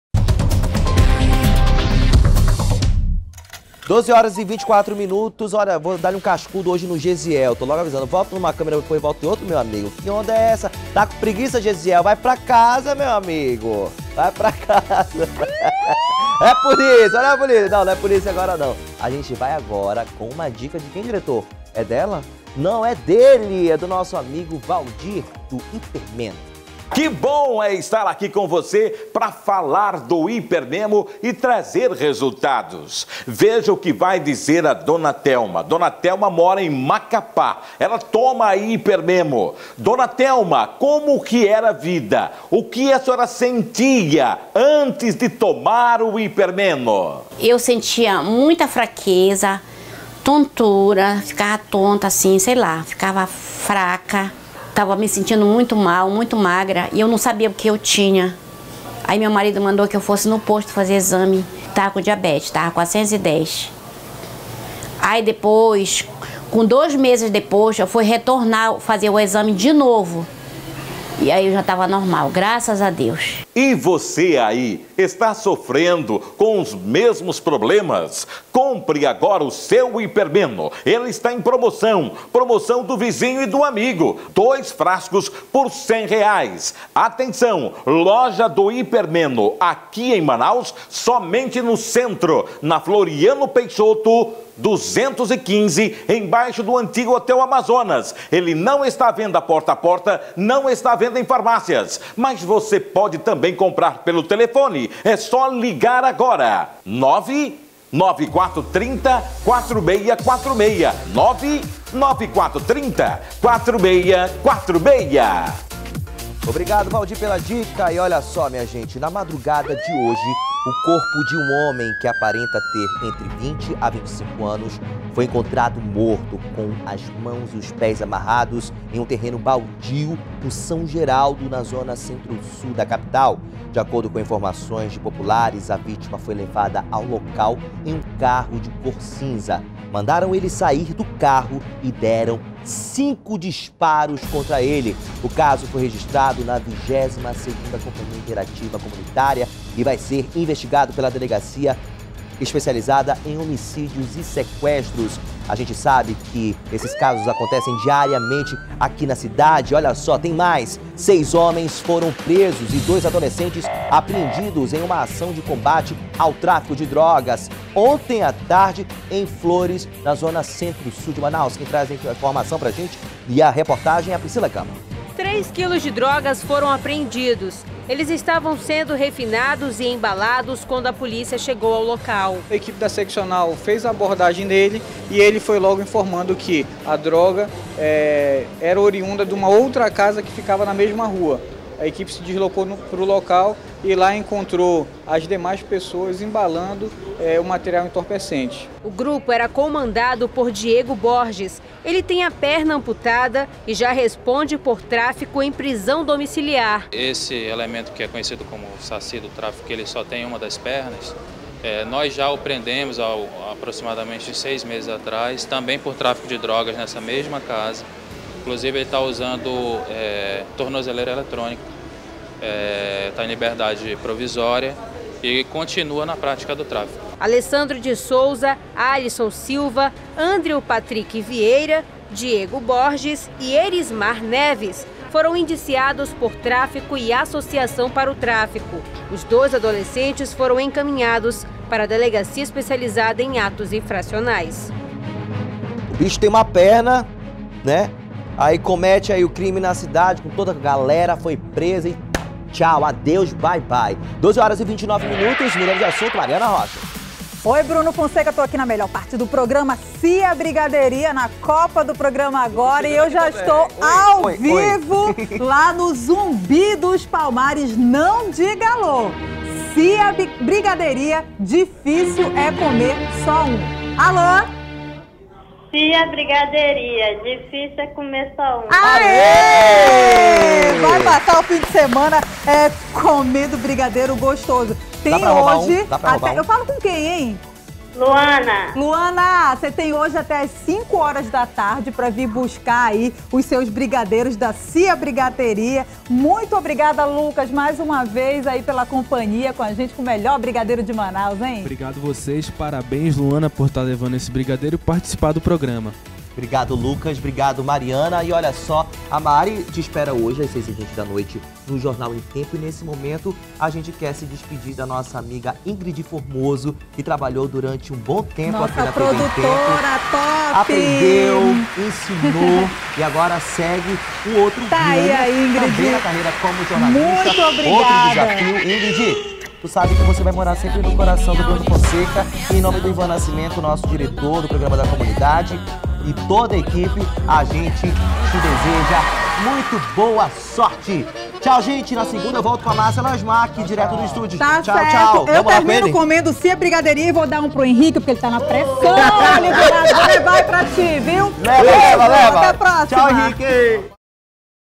Quadrilha é presa e 2 adolescentes apreendidos suspeitos de tráfico de drogas
05º BLOCO AGORA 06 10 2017 - Quadrilha é presa e 2 adolescentes apreendidos suspeitos de tráfico de drogas